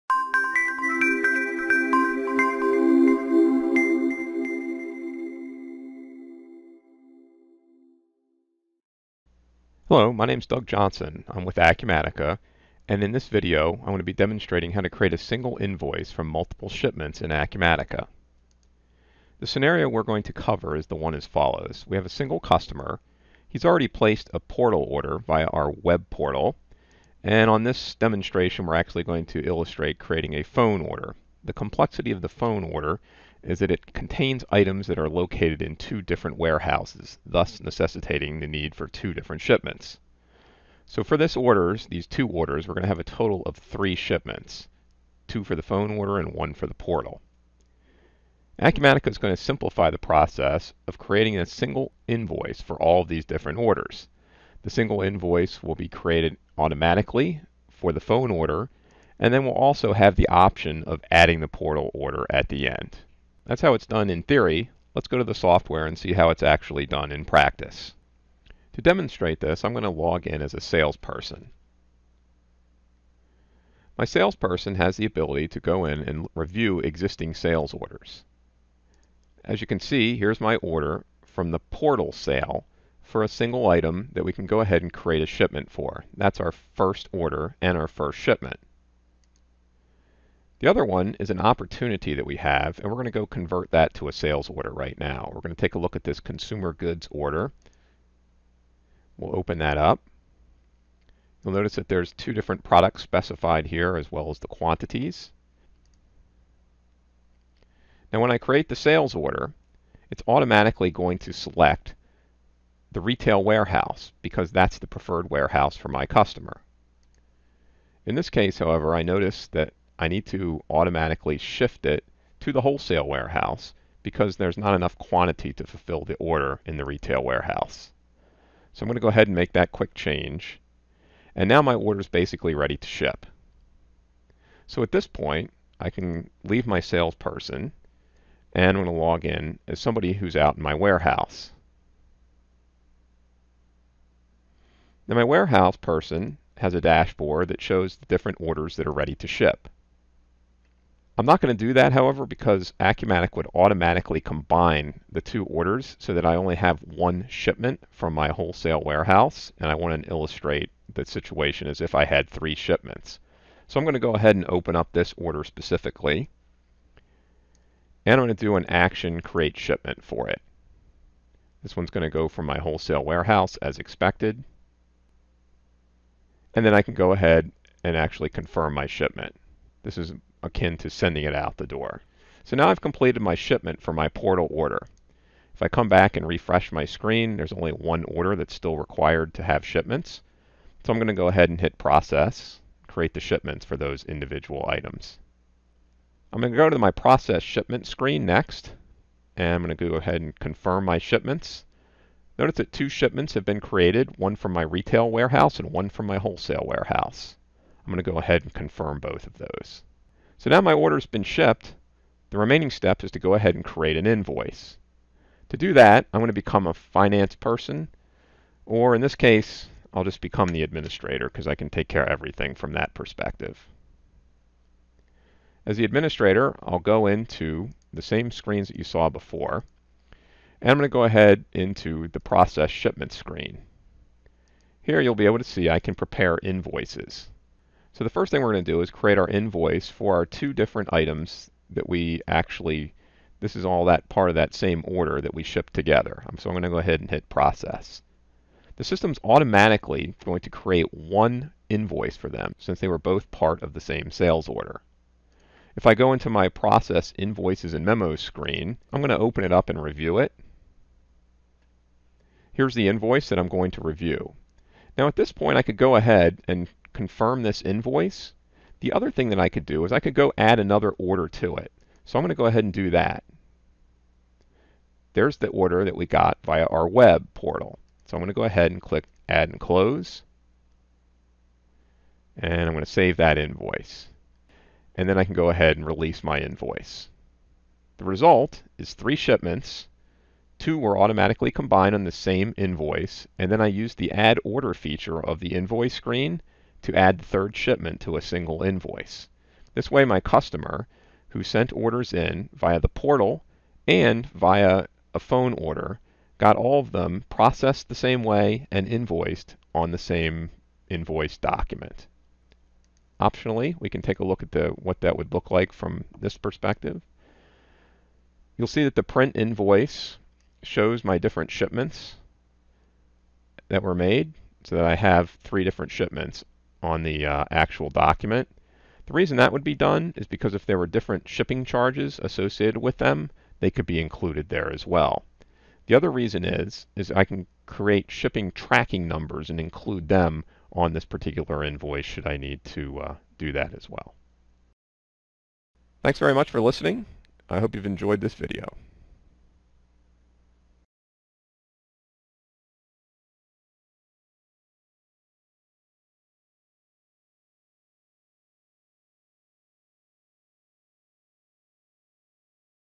Multiple Shipments - 1 invoice
Demonstration of a salesperson verifying a phone order and taking a web order. Next the shipping manager created 3 shipments (one for the web order and 2  from different warehouses for the phone order). Next accounting creates a single invoice showing all three shipments. Sounds complex, but it's easy using Acumatica.